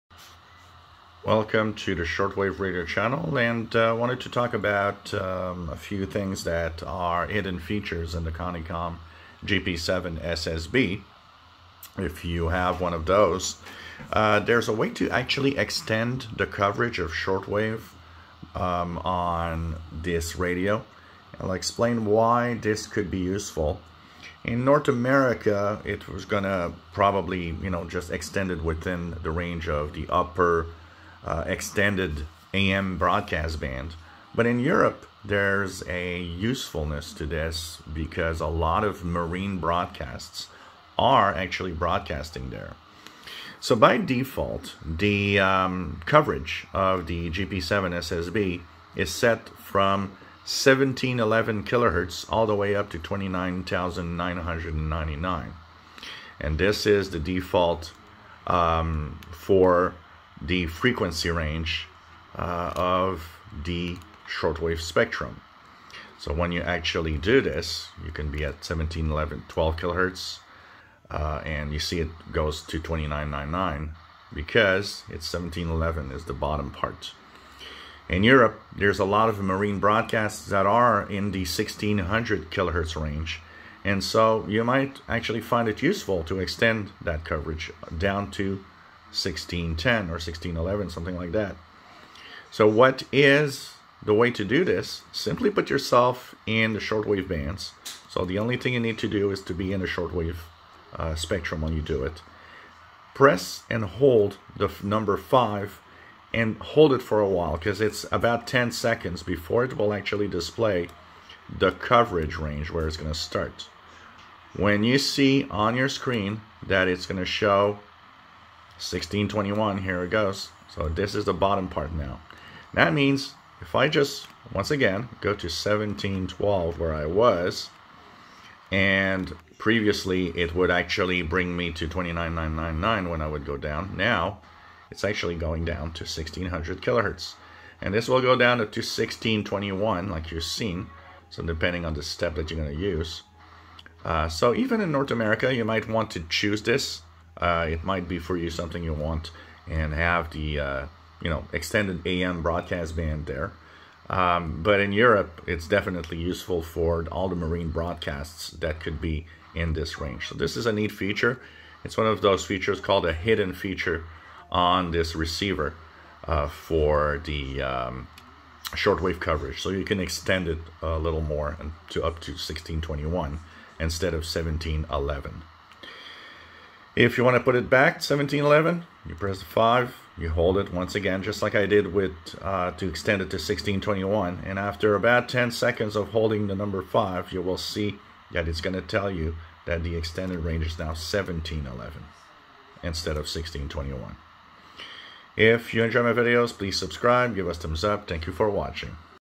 Hidden Feature How to extend the coverage of Shortwave on CountyComm GP-7/SSB Tecsun PL-368
You can change the start of shortwave coverage from 1711 to 1621 kHz , useful in Europe where a lot of Maritime stations are